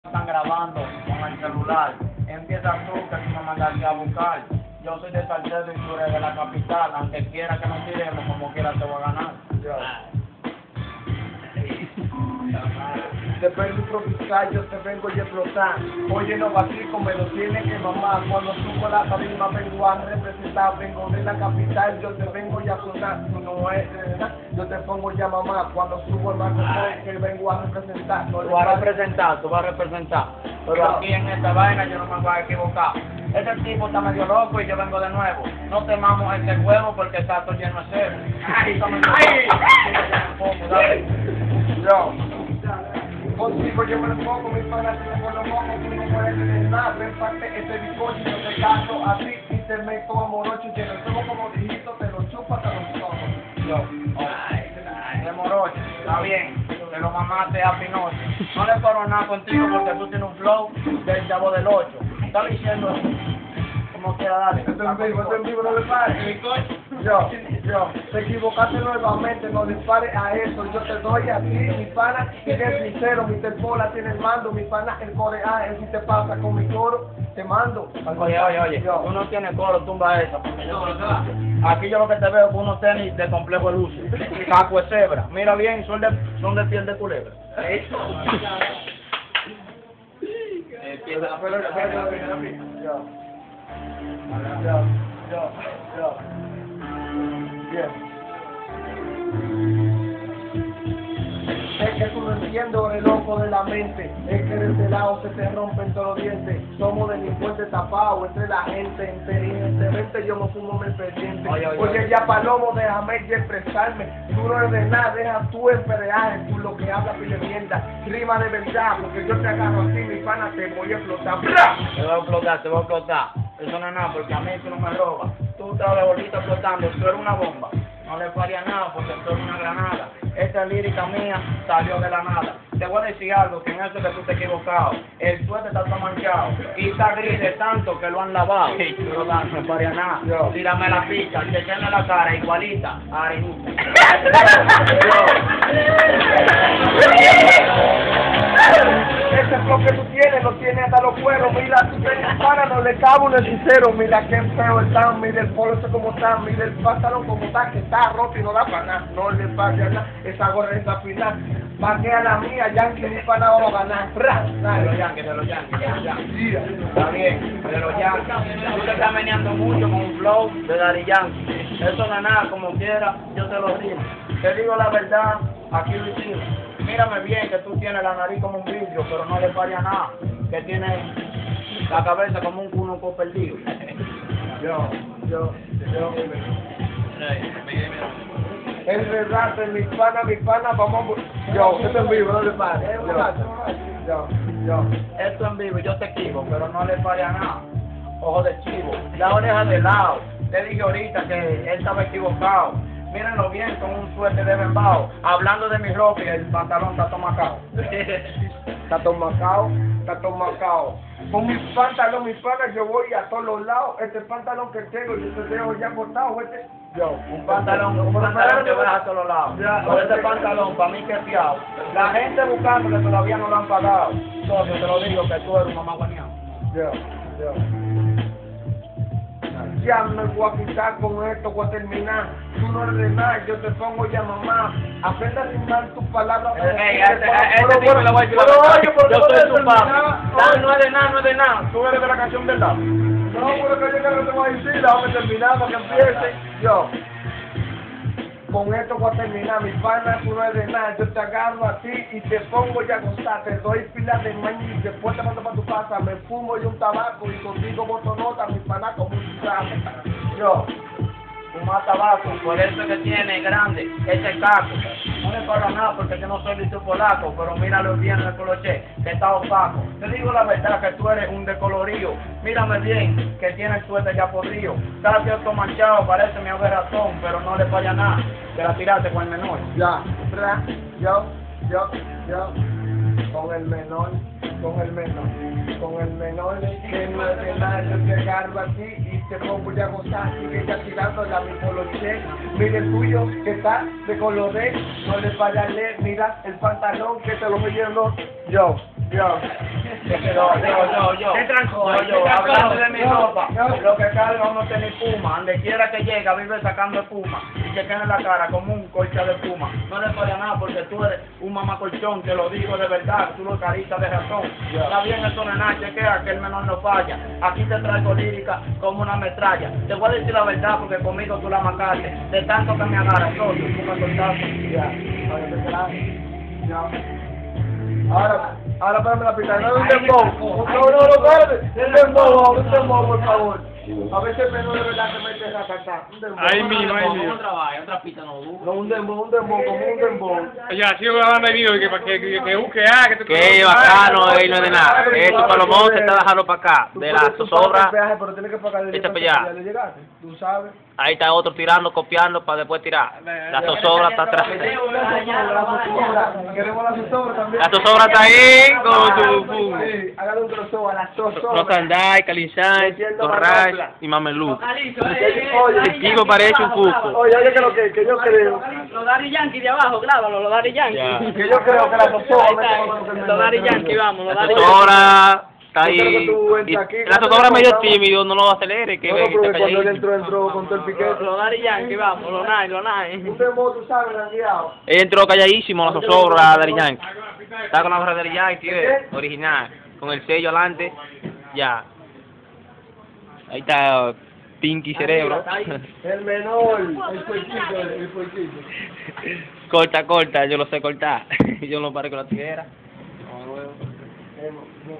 El Menor Vs El Capitaleño (EN LA FAMA INTER FLASH)
tirandose fuerte mente. Ayala - Lito - Rafa en el Coro y Dionel en Camara.